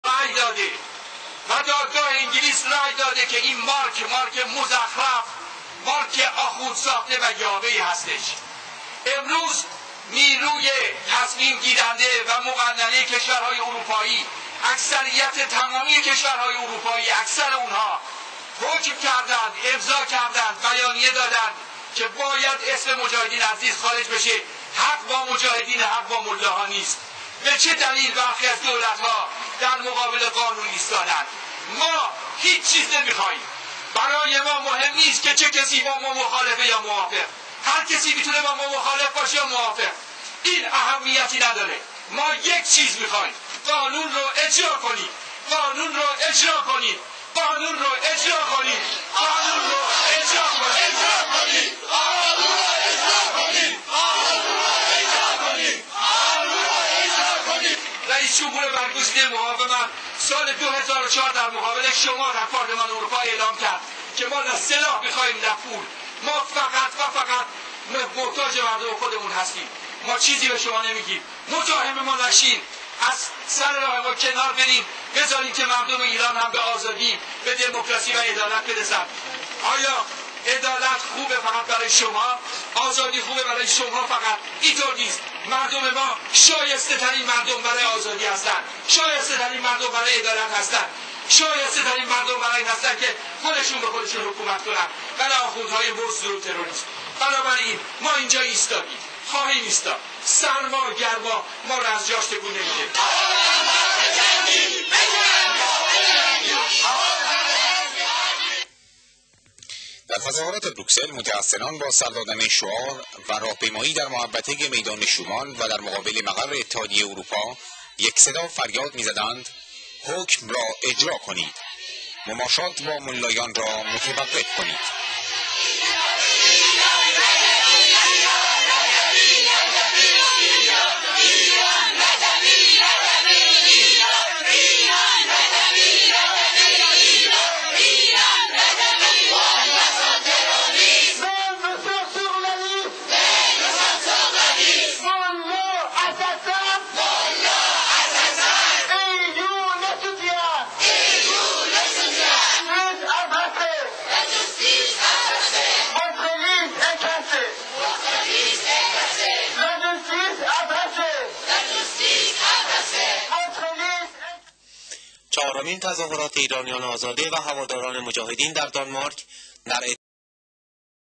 Mehdi Abrishamchi: Ghanoon ro ejra konid!
Mehdi Abrishamchi (Brussels):

Dadgahe Europa ray dade va dadgahe engilis ray dade ke in mark (terroristi), marke mozakhraf, va marke akhoond sakhtey hastesh.

Nirooye tasmimgirande va moghadane keshvarhaye europai, aksariyate tamamiyeh keshvaraye europai, hokm kardand, emza kardand, bayaniyeh dadand, ke bayad ezme mojahedin az list kharey beshe. Hagh ba mojahedine, hagh ba mollaha nist. Be che dalil barkhi az dolatha dar moghalebe ghanoon istadand.

Ma hich chiz nemikhajm. Baraye ma mohem nist ke che kesi ba ma mokhalefe ya moavegh! Harkesi mitoone ba ma mokhalef bashe ya moafegh. Ma yek chiz mikhajm, GHANOON RO EJRA KONID!

Raisjomhore moghavemat sale 2004 dar moghalebe shoma, dar parleman europa elam kard ke ma na selah mikhajm na pool. Ma chizi be shoma nemigim. Mozaheme ma nashin! Az sare rahe ma kenar berid. Bezar ke mardome Iran ham be azadi, democracy va edalat beresan. Aya edalat khoobe, faghat baraye shoma? Aya azadi khoobe baraye shoma faghat? INTOR NIST. Mardome ma, shayeste tarin mardom baraye azadi hastand. Shayeste tarin mardom baraye edalat hastand.

Banabarin, ma inja istadim, khahim istad, sarma garma, maro az jash tekoon nemikhore